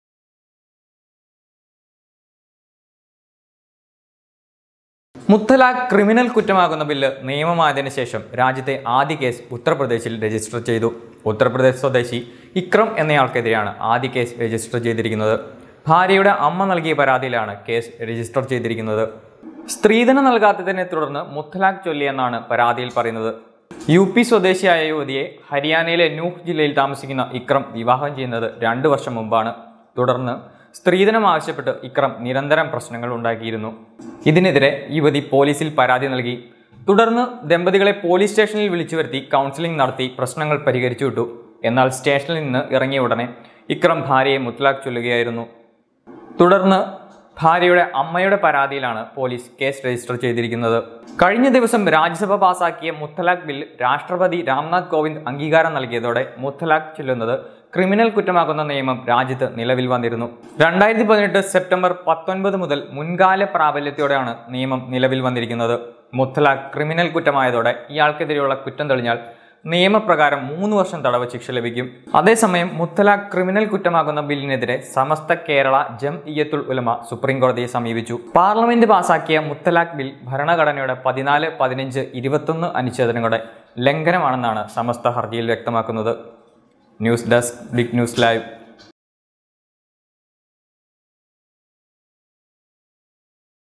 രാജ്യത്തെ ആദ്യ മുത്തലാഖ് കേസ് ഉത്തര്‍പ്രദേശില്‍;-First triple talaq case registered in utter pradesh
മുത്തലാഖ് ക്രിമിനല്‍ കുറ്റമാകുന്ന ബില്ല് നിയമമായതിന് ശേഷം രാജ്യത്തെ ആദ്യ കേസ് ഉത്തര്‍പ്രദേശില്‍ രജിസ്റ്റര്‍ ചെയ്തു.